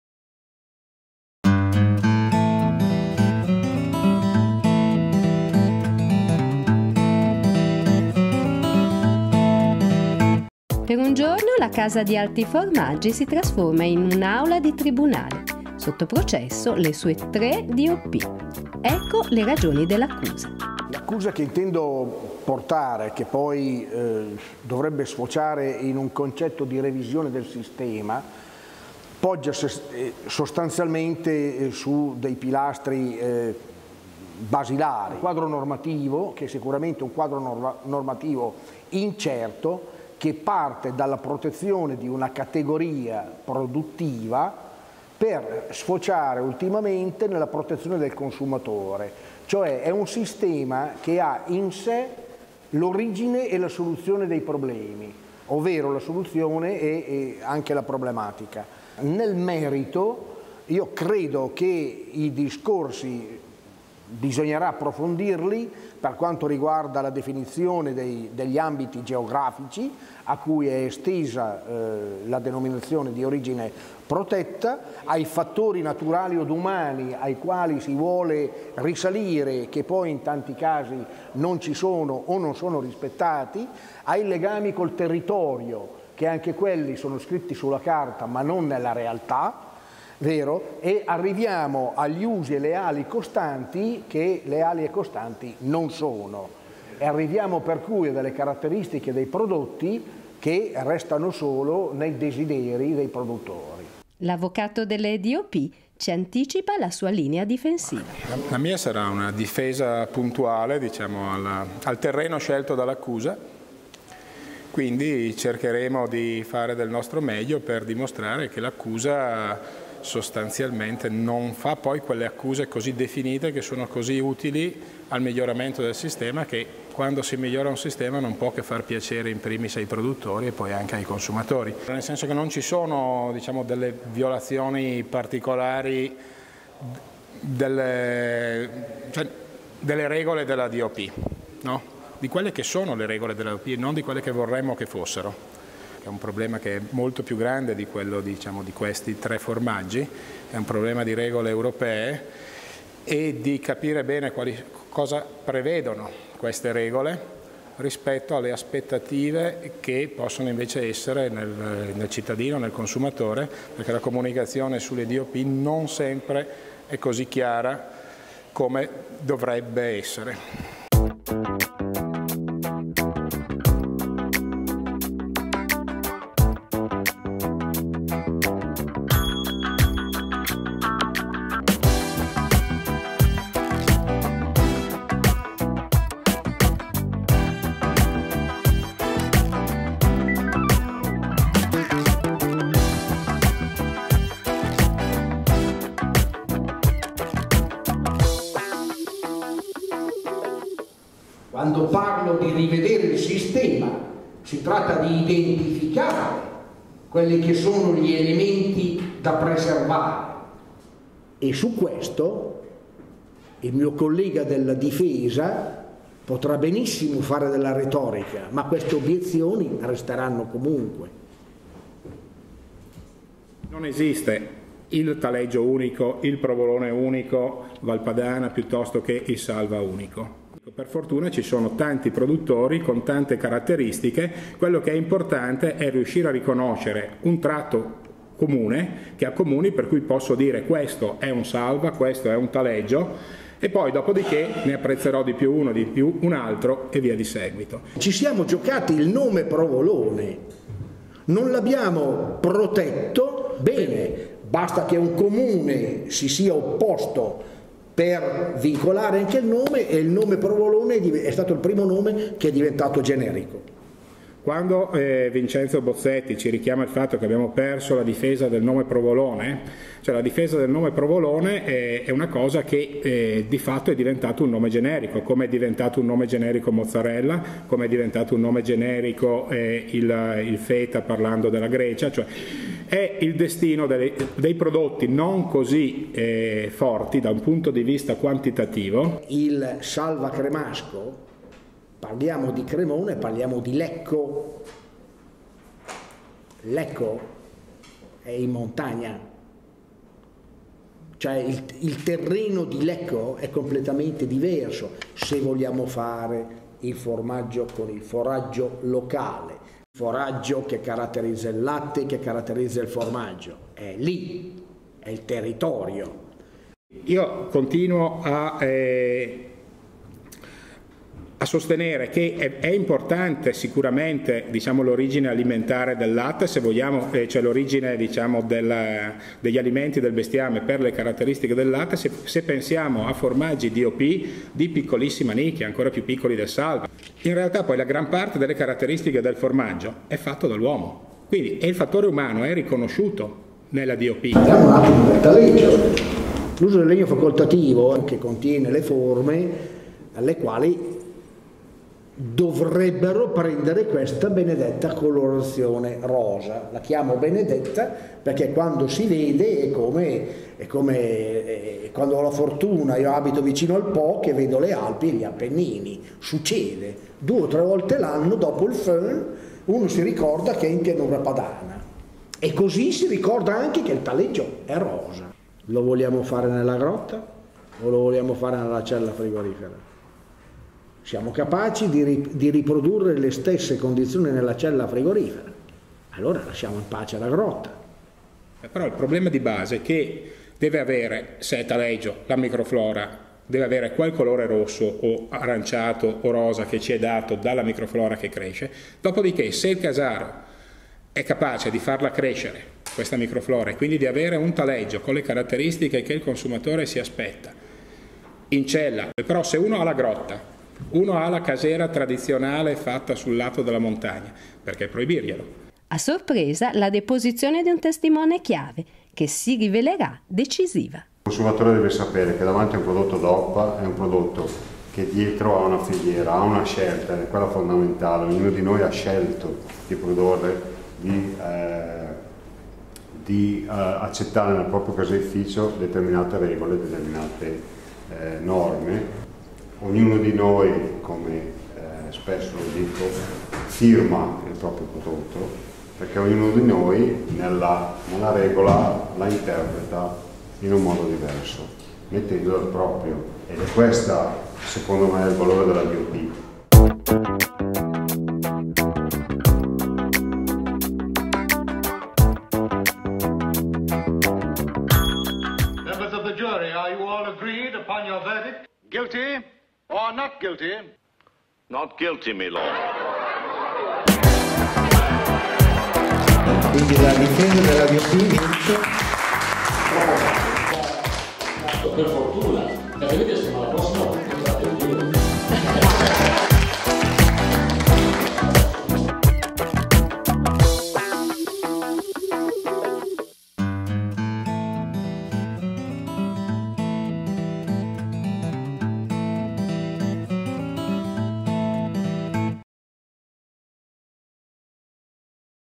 10 febbraio 2014 - Processo ai 3 formaggi DOP
Il 10 febbraio si è svolto il processo ai 3 DOP: Taleggio, Salva Cremasco e Provolone Lombardo. L'accusa alle 3 DOP è stata redatta da Vincenzo Bozzetti, tecnico del settore lattiero caseario e giornalista, mentre la difesa è stata assunta dal Prof. Germano Mucchetti.